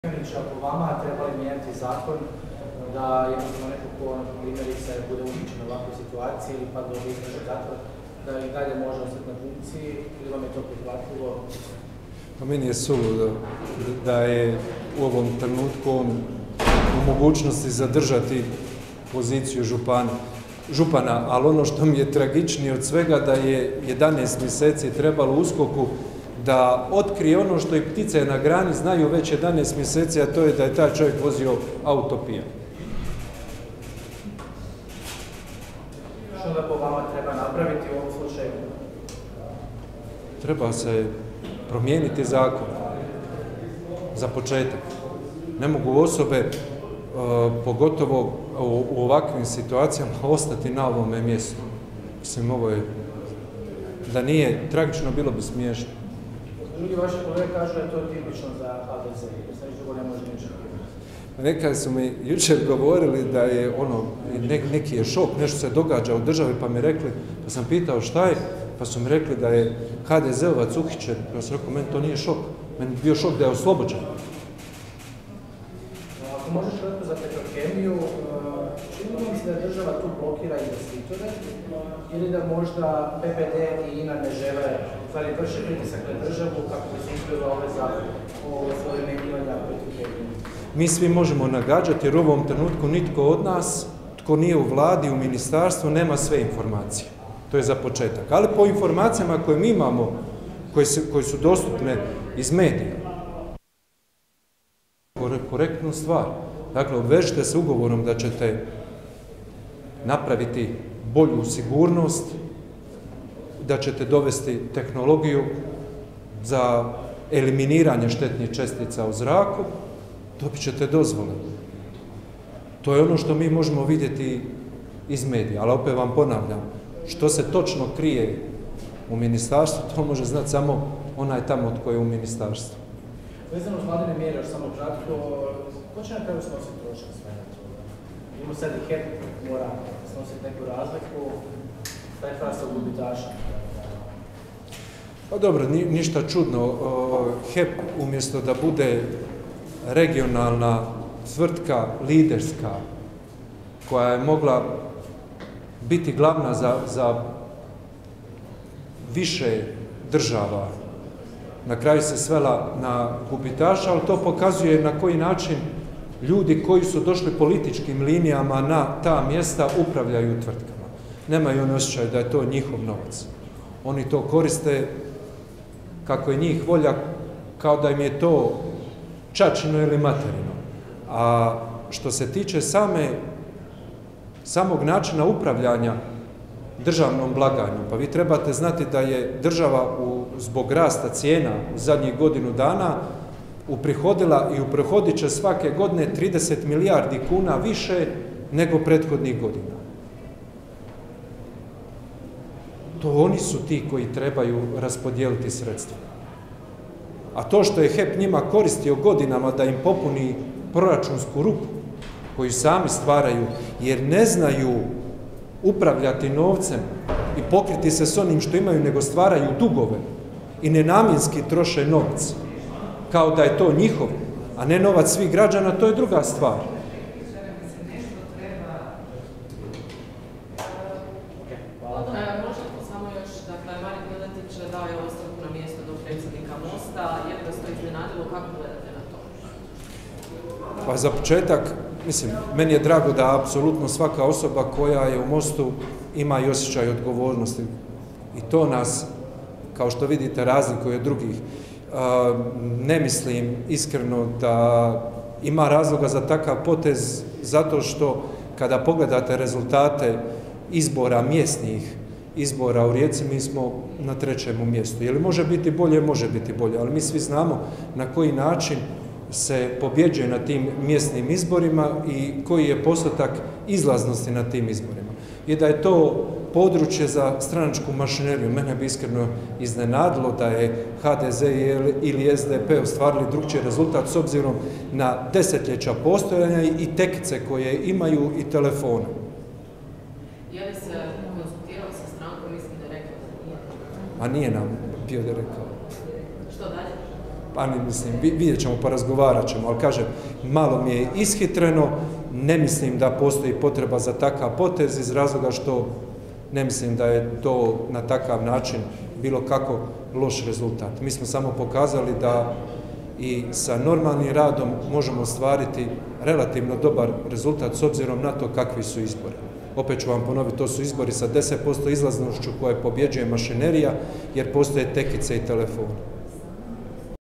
Petrov o Dekaniću: "Nije šok da je HDZ-ovac uhićen, bio bi šok da je oslobođen"
Na konferenciji za medije odrzanoj u Saboru aktualne teme komentirao je zastupnik Bozo Petrov